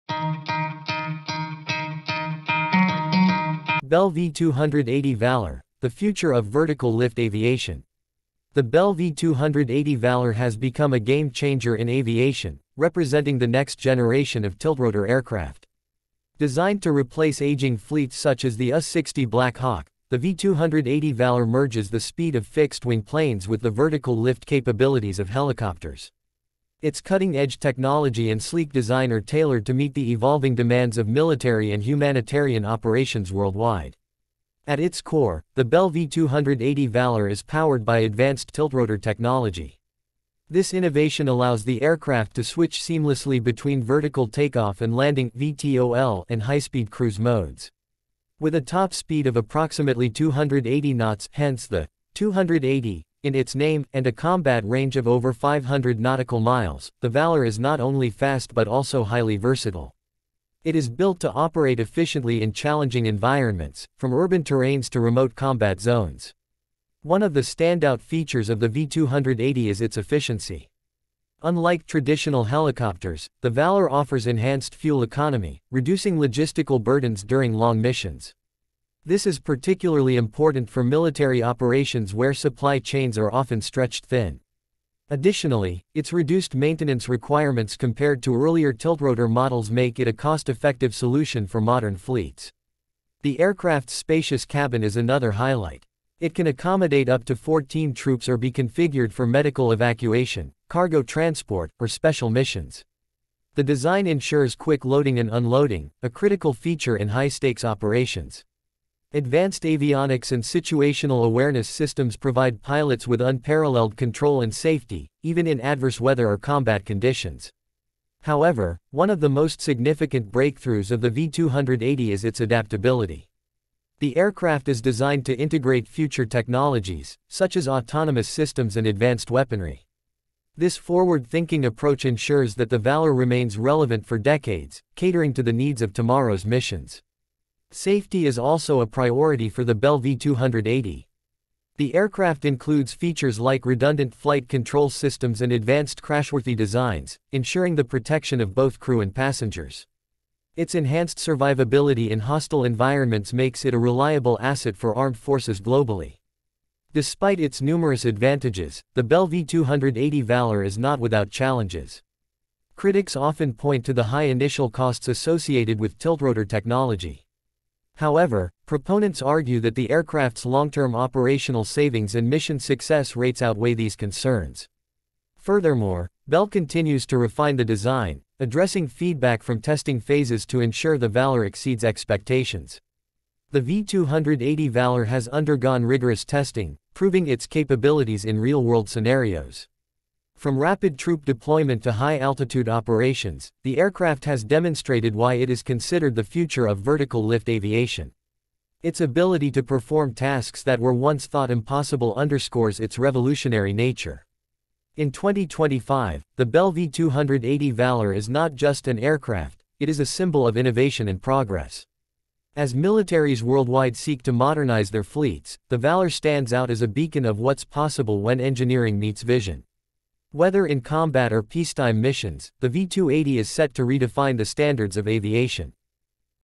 Bell V-280 Valor: Revolutionizing Aviation in 2025"
Discover the cutting-edge Bell V-280 Valor, a next-generation tiltrotor aircraft redefining speed, efficiency, and versatility. Designed for modern military and humanitarian missions, the V-280 combines the agility of helicopters with the power of fixed-wing aircraft. Learn how this game-changing innovation is setting new standards in vertical lift aviation.